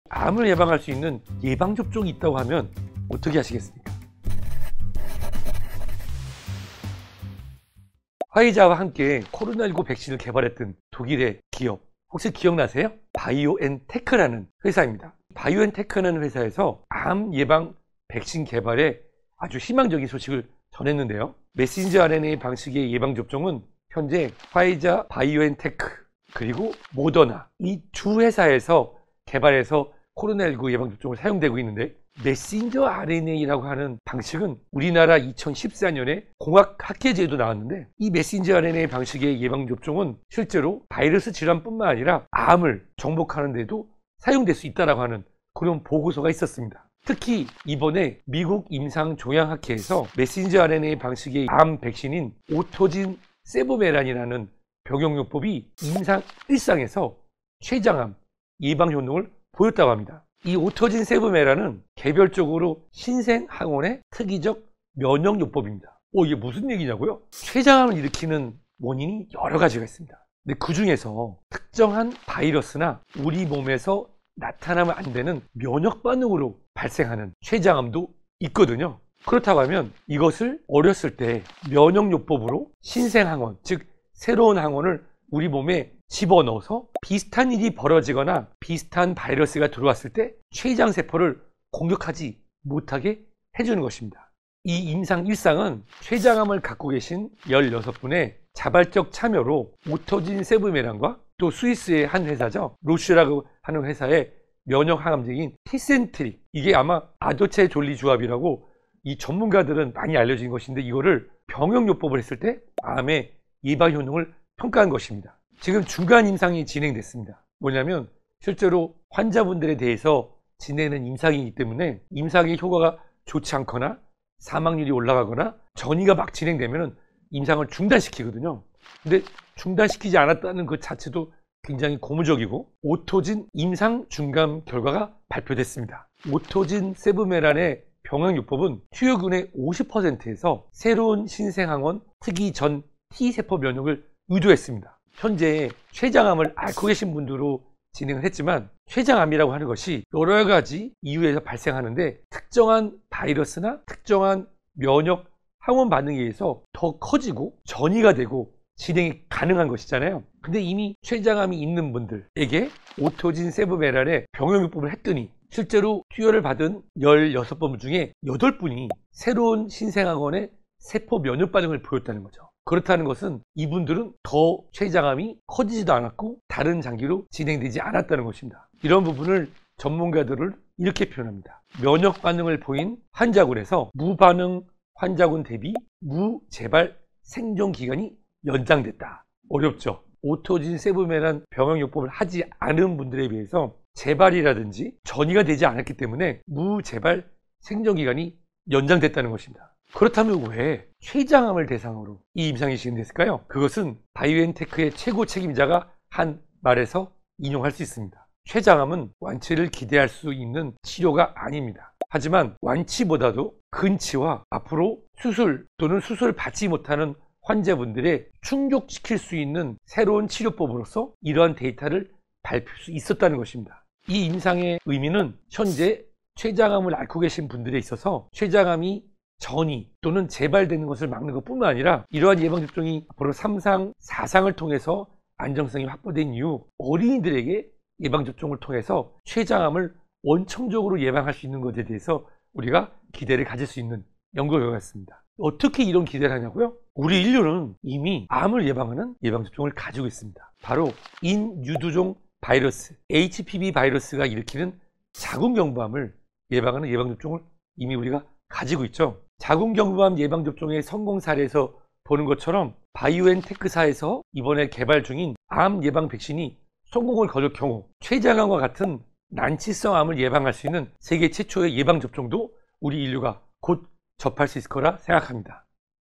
암 정복학 개론 ep10. 췌장암 예방접종 개발!? 난치성 암 정복할 수 있을까? [닥터유의 건강이야기]
화이자와 함께 코로나19 백신을 개발했던 독일의 기업, 혹시 기억나세요? 바이오엔테크라는 회사입니다. mRNA의 코로나19 예방접종으로 유명한 회사죠. mRNA라고 하는 방식은 우리나라 2014년에 공학 학계지에도 나왔는데 이 mRNA방식의 예방접종이 실제로 바이러스 질환뿐만 아니라 암을 정복하는데도 사용될 수 있다라고 하는 그런 보고서가 있습니다.